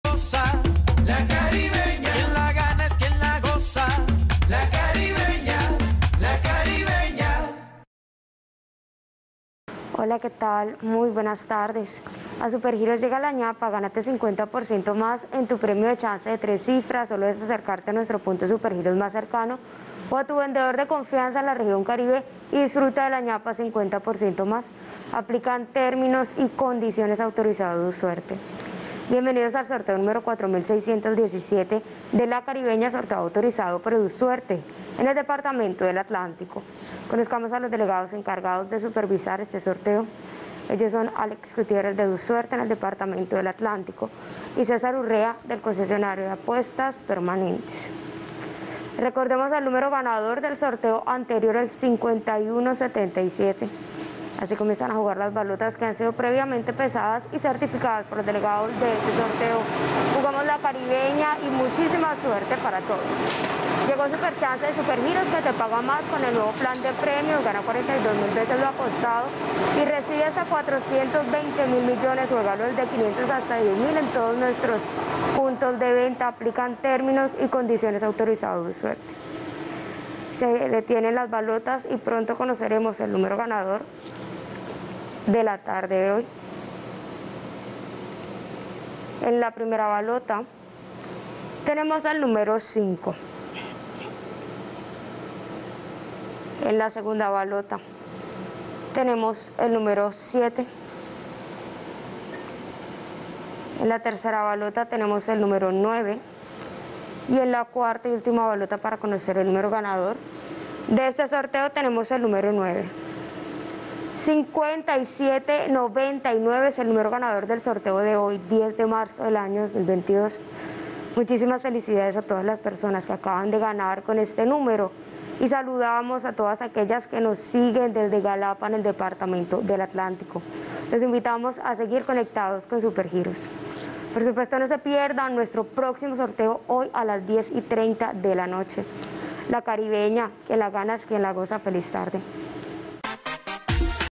CARIBEÑA DÍA Resultado del JUEVES 10 de marzo de 2022 ✅🥇🔥💰
Hola buenas tardes, te presentamos los resultados de las loterías y chances en Colombia, vamos con el Resultados del Chance Caribeña día del JUEVES 10 de marzo de 2022.